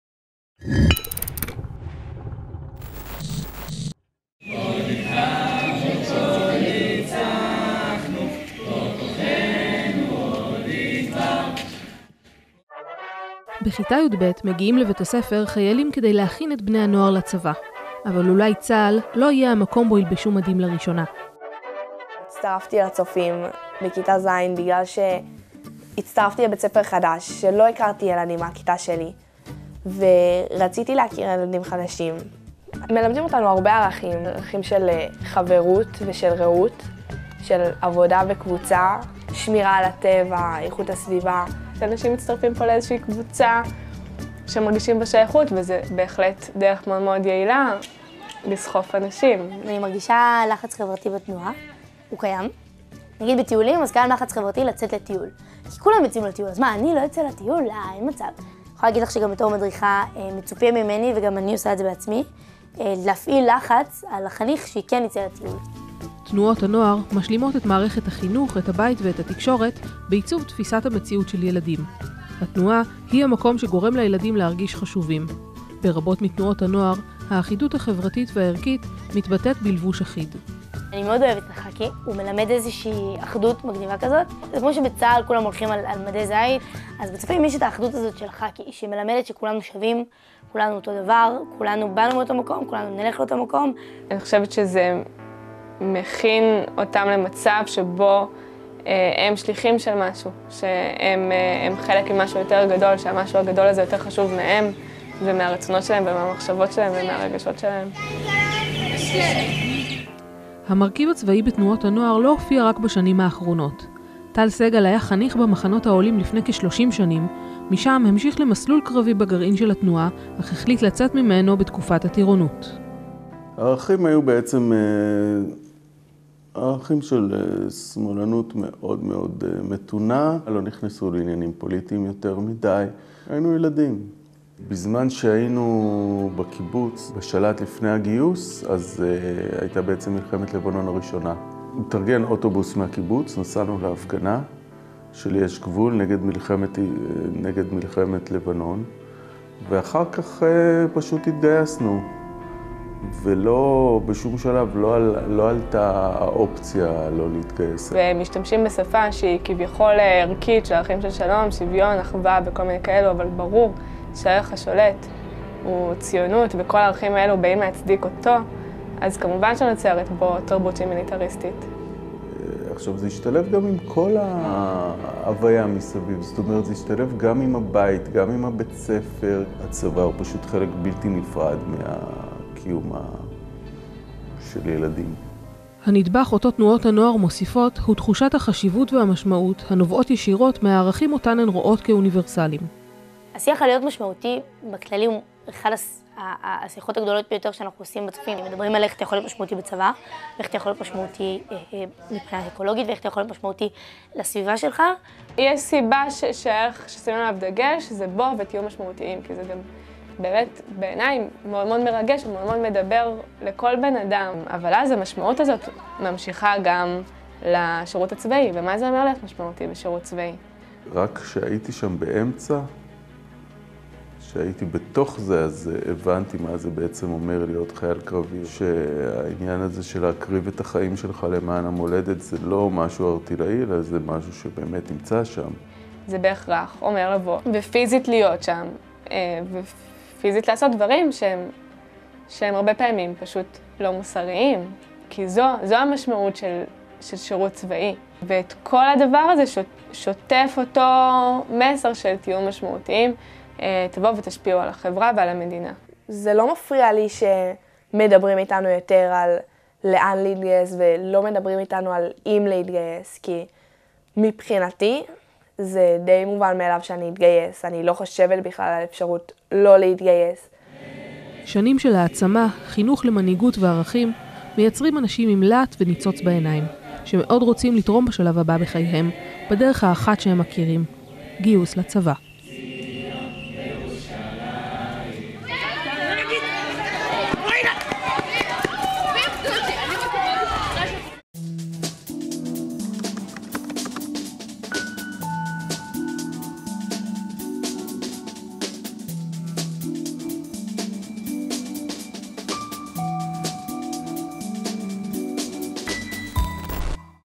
Militarism in Youth movements
Discourse on the soldiers, heroism and sacrifice increases on Memorial Day. But where these values come from? Some boys and girls who grew up to be soldiers, were educated in youth movements. What is the connection between youth movements and the army?